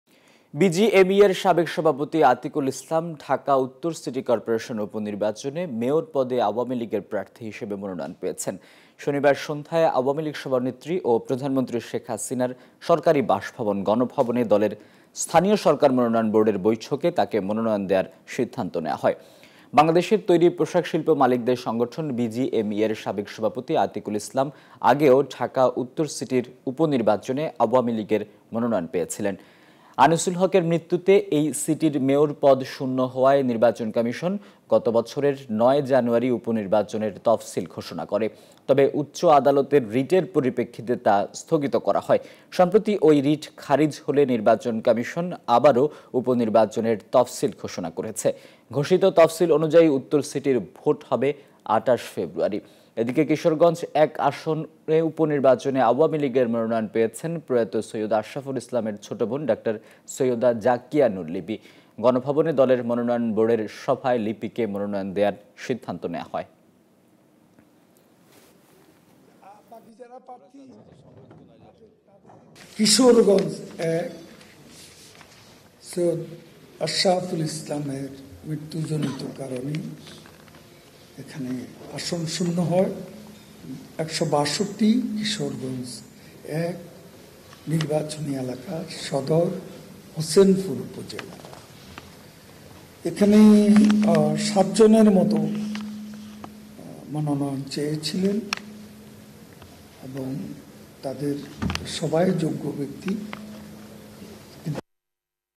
আতিকুল ইসলাম -ঢাকা উত্তর সিটি করপোরেশন উপনির্বাচনে মেয়র পদে আওয়ামী লীগের প্রার্থী 27Jan.19
বিজিএমইএর সাবেক সভাপতি আতিকুল ইসলাম ঢাকা উত্তর সিটি করপোরেশন উপনির্বাচনে মেয়র পদে আওয়ামী লীগের প্রার্থী হিসেবে মনোনয়ন পেয়েছেন।
শনিবার সন্ধ্যায় আওয়ামী লীগ সভানেত্রী ও প্রধানমন্ত্রী শেখ হাসিনার সরকারি বাসভবন গণভবনে দলের স্থানীয় সরকার মনোনয়ন বোর্ডের বৈঠকে তাকে মনোনয়ন দেওয়ার সিদ্ধান্ত হয়। বাংলাদেশের তৈরি পোশাক শিল্প মালিকদের সংগঠন—বিজিএমইএ’র সাবেক সভাপতি আতিকুল ইসলাম আগেও ঢাকা উত্তর সিটির উপনির্বাচনে আওয়ামী লীগের মনোনয়ন পেয়েছিলেন।

News24 Official Address:
NEWS24
371/A
Bashundhara Road
Block – D
Bashundhara Residential Area
Dhaka - 1229